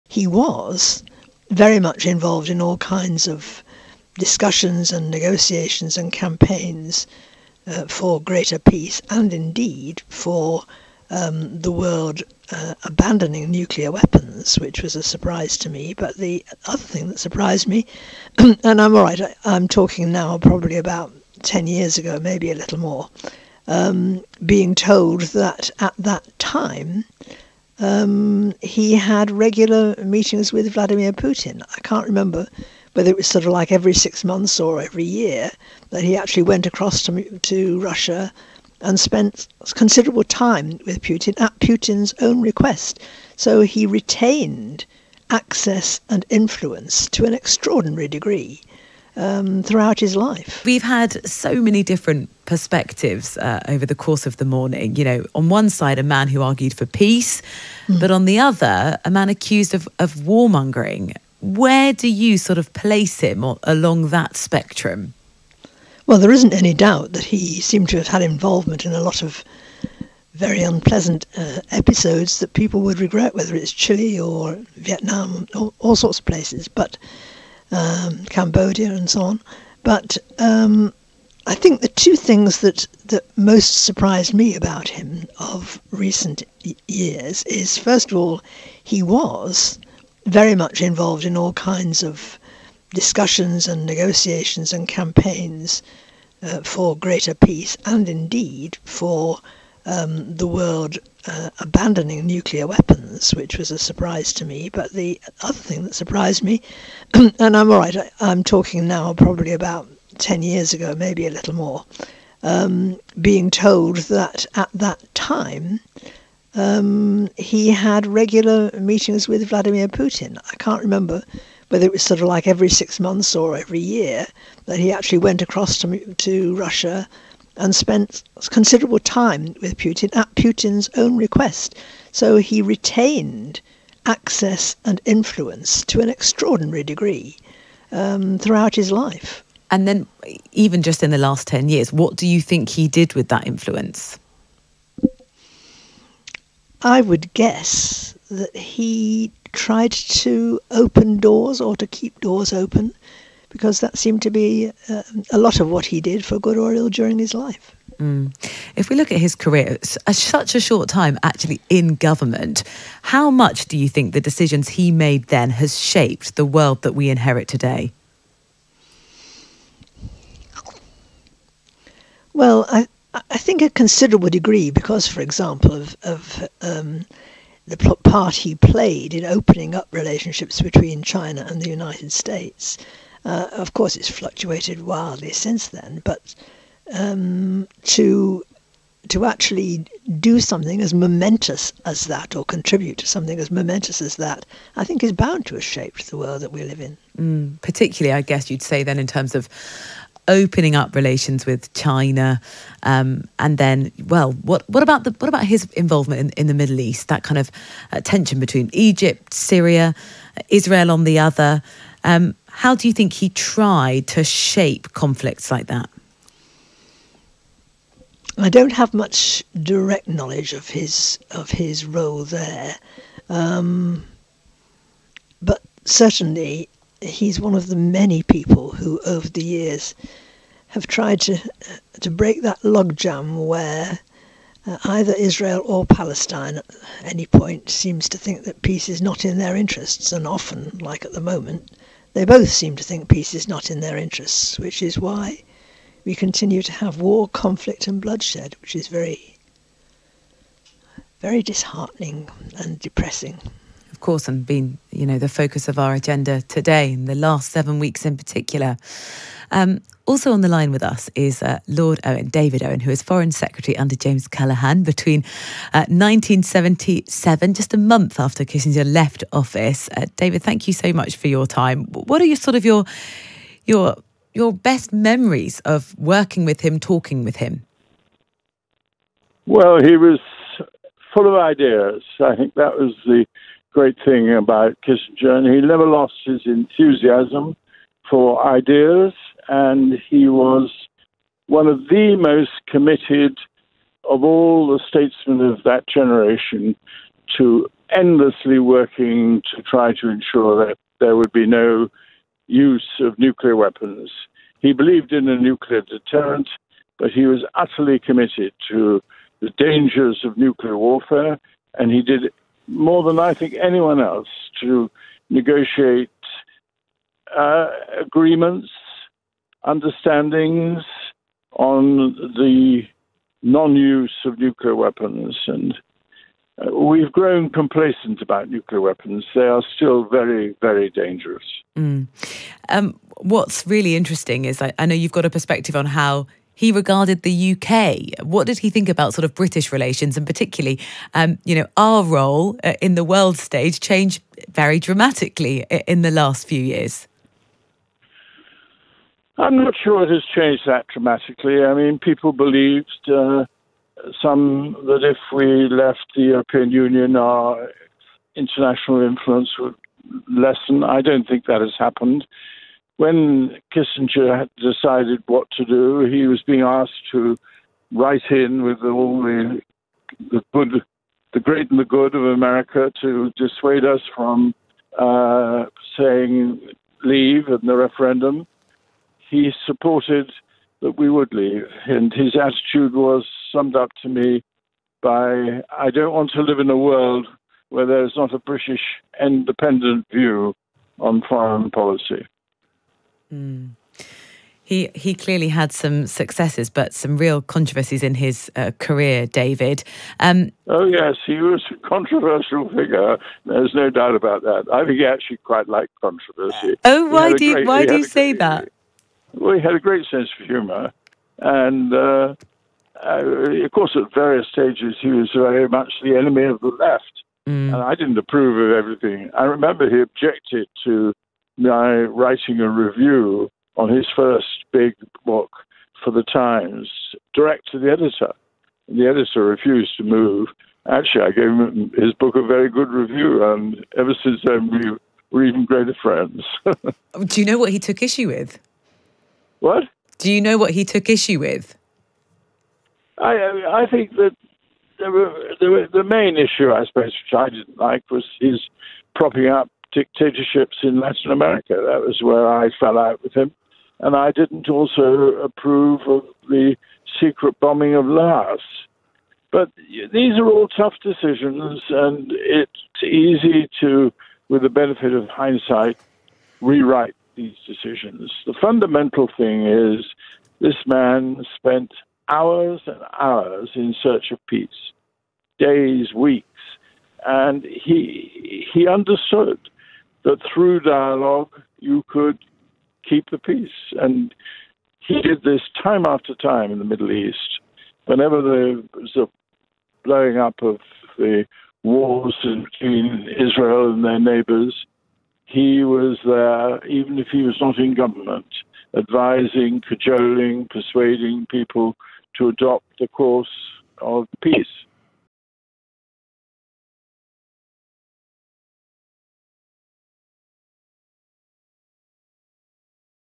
Henry Kissinger spent 'hours and hours in search of peace' | Lord Owen and Dame Margaret Beckett
“He understood that through dialogue you could keep the peace. And he did this time after time in the Middle East, whenever the blowing up of the walls between Israel and their neighbours, he was, even if he was not in government, advising, cajoling, persuading people to adopt the course of peace.”

Former foreign secretaries, Lord Owen and Dame Margaret Beckett look back at the life and career of former US secretary of state Henry Kissinger.